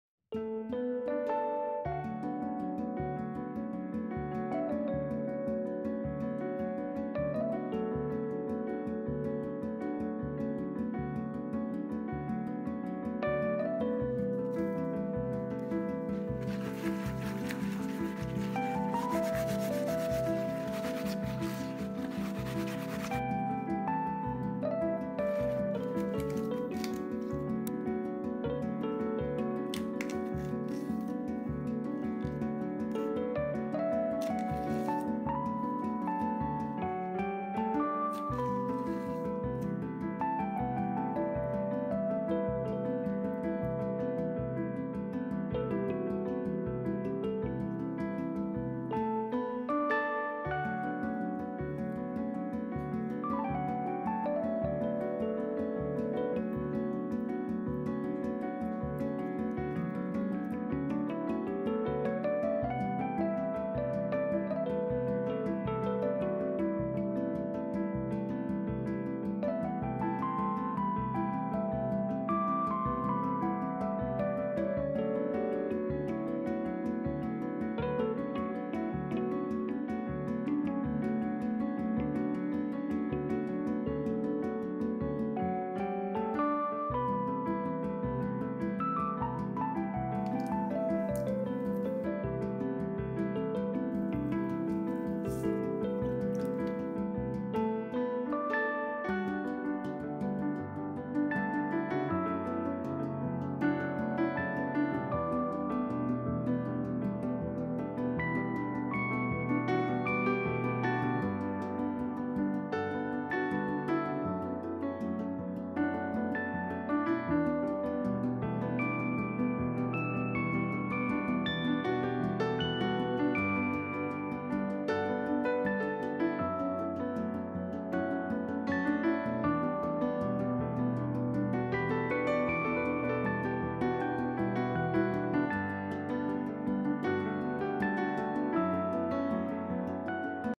【🔥電飯煲】⭐簡易蒸餸🦐秋葵釀蝦滑💕家常菜🍚又鮮又嫩🥘連飯一齊煮🌙晚餐料理😍簡易又快速✨懶人廚房
味道又鮮，秋葵也嫩，大家不妨嘗試一下喔！